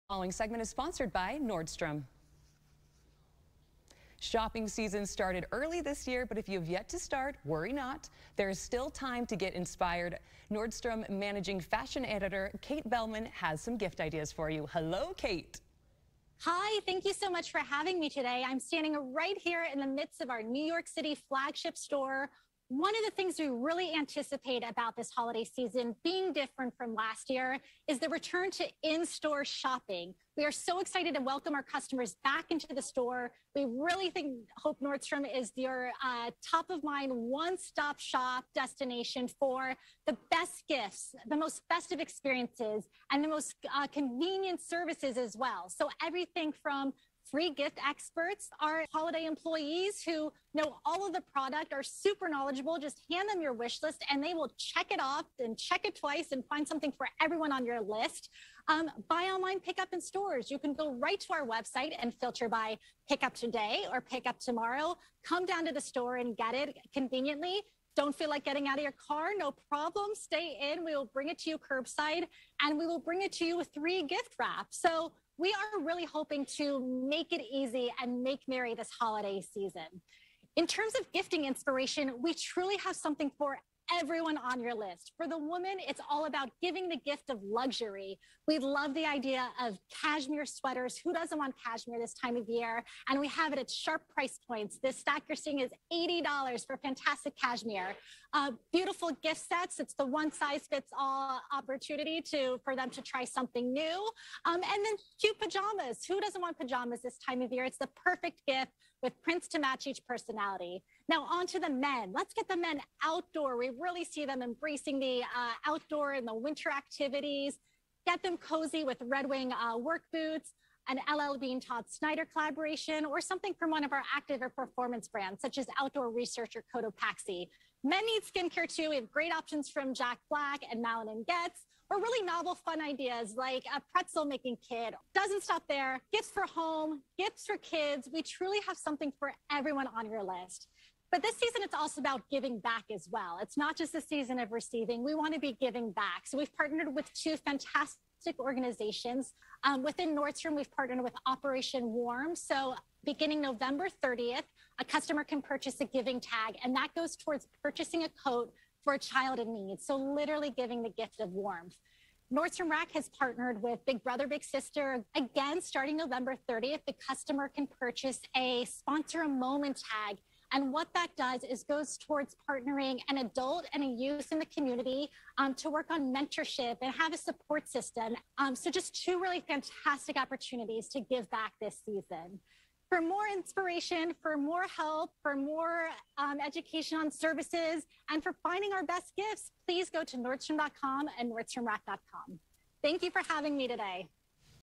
Gift guide from Nordstrom
Nordstrom managing fashion editor, Kate Bellman joined Fresh Living with great gift ideas.